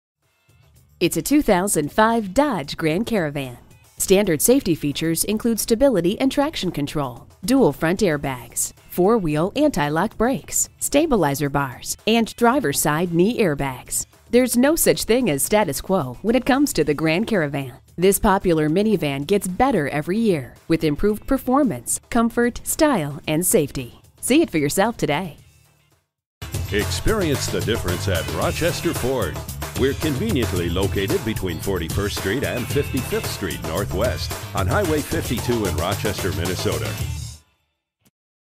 2005 Dodge Grand Caravan Rochester MN Winona, MN FA158406 - SOLD
Year: 2005
Make: Dodge
Model: Grand Caravan
Trim: SE
Engine: 3.3L V6 OHV
Transmission: 4-Speed Automatic
Color: White
Mileage: 175759
Stock #: FA158406
VIN: 1D4GP24R45B422530

Address:
4900 Hwy 52 North